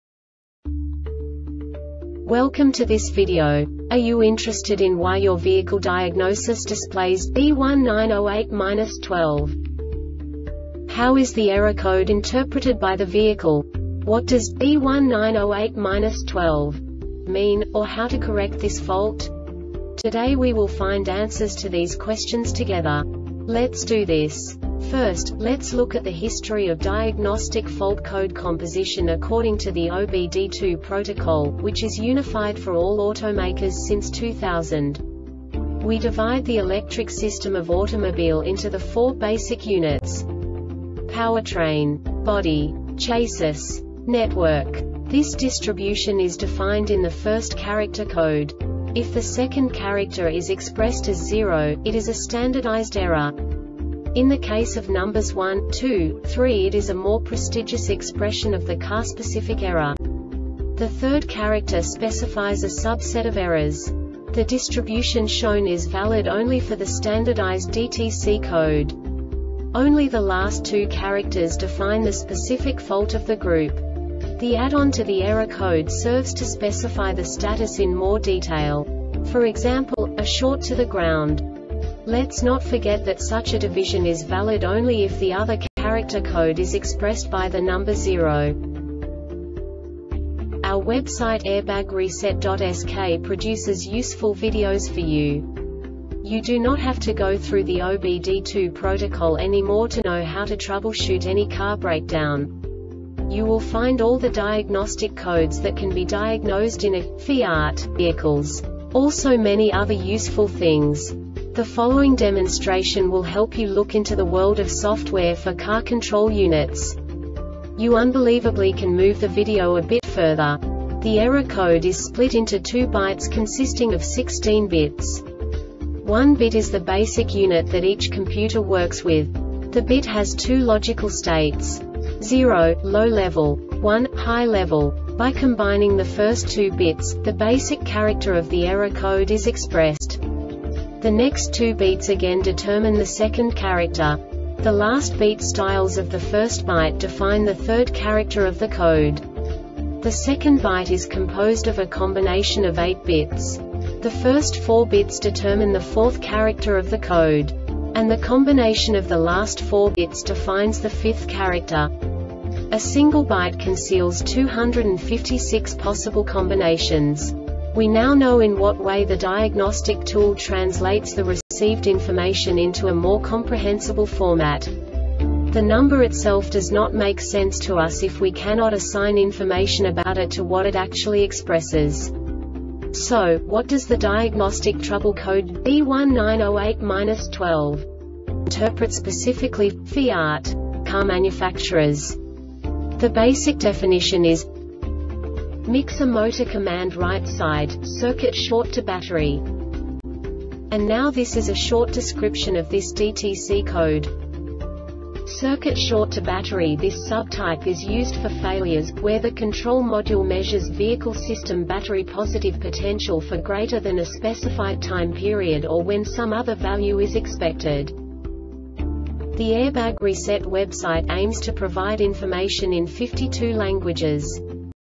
DTC Fiat B1908-12 Short Explanation
Contents:
0:21 Basic DTC analysis according to OBD2 protocol standard.
1:48 Insight into programming
2:58 A diagnostically understandable description of the Diagnostic Trouble Code
3:05 Basic error definition

Make:
FIAT
Code:
B1908-12
Definition:
Mixer Motor Command Right Side - Circuit Short To Battery
Failure Type: